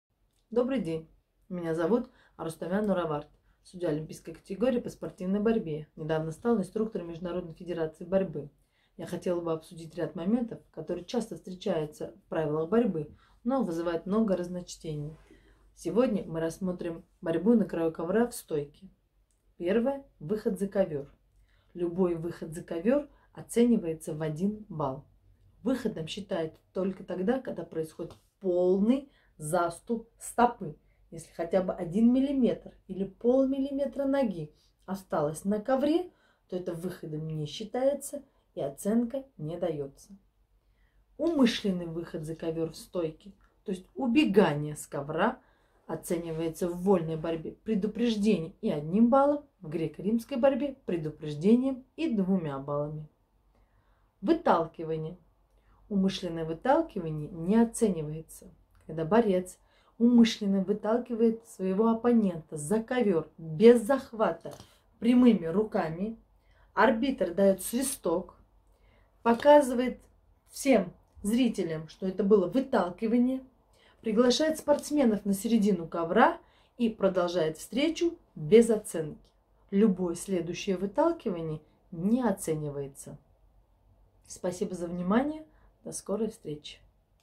Правила борьбы с Норавард Арустамян
Сейчас, когда борцовский календарь приостановлен, самое время закрепить знания правил нашего вида спорта, которые в последние годы менялись десятки раз.

ФСБР открывает новую рубрику, которую будет вести Инструктор Объединенного мира борьбы Норавард Арустамян. В первом выпуске она расскажет о том, за что даются баллы, когда борьба происходит в стойке на краю ковра.